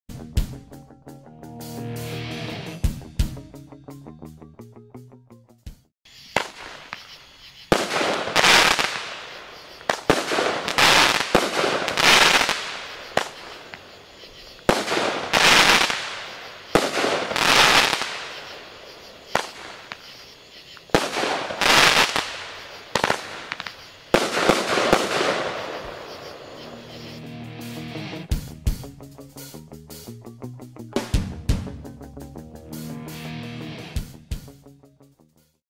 Legend Fireworks - Key Break 200 Gram
Key Break by Legend Fireworks - Red dahlia with time rain, green dahlia with time rain, Finale with brocade mines to brocade crown.